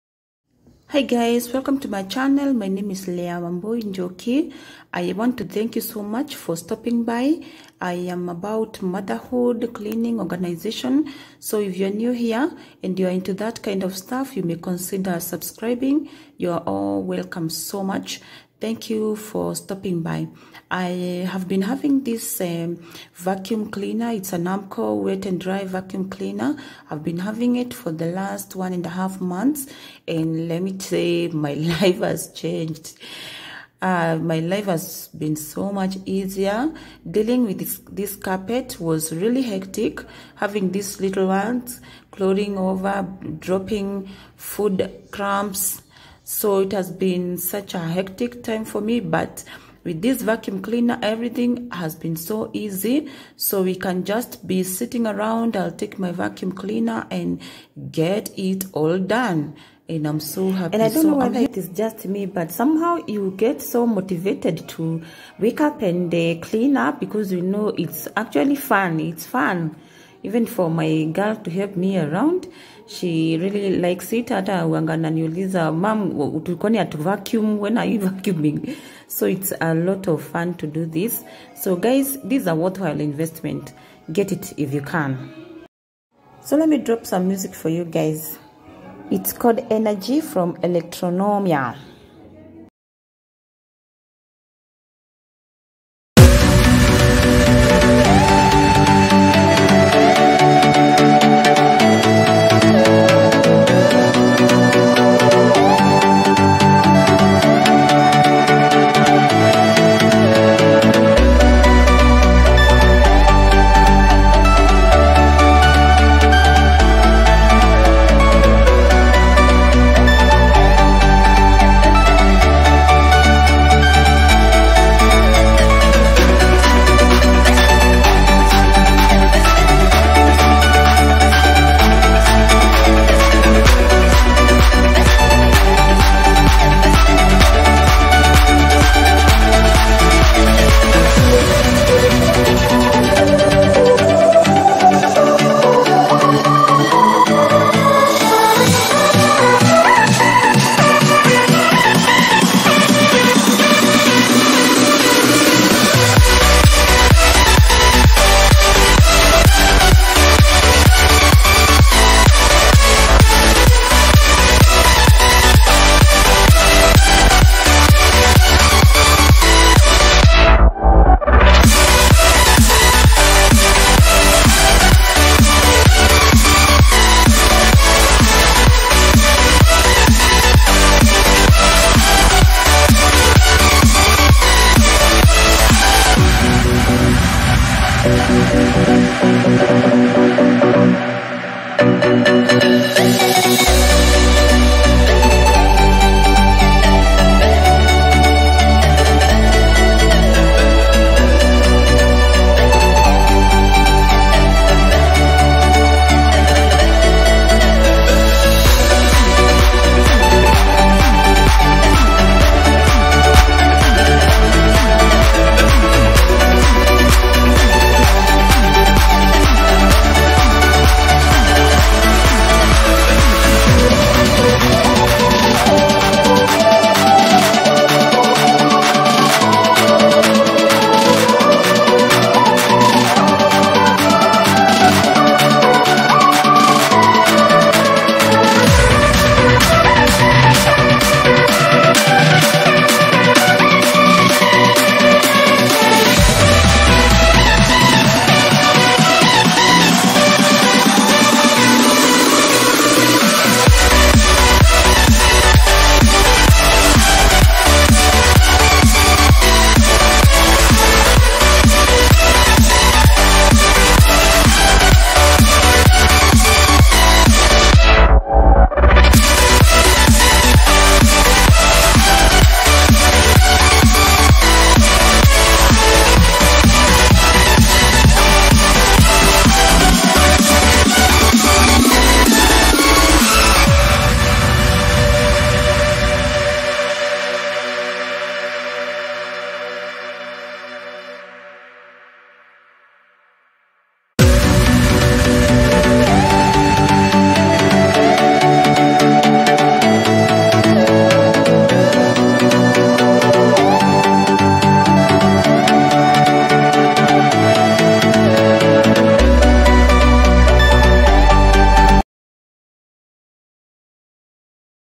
ARMCO WET AND DRY VACUUM CLEANER REVIEW/REVIEW VACUUM CLEANER 2021/VACUUM CLEANER KENYA/
In this video i review Armco Wet and Dry Vacuum Cleaner. This vacuum cleaner is a wet and dry vacuum cleaner and one of the best wet and dry vacuum cleaners in Kenya. So guys this is a kenyan vacuum cleaner review in the year 2021. I promised a review on this  vacuum cleaner so here we are.

Its has made cleaning so much easier for me and i love it. Thanks a lot for watching.

0:00 Intro
1:10 Ooh my baby .

God Bless you all . Hugs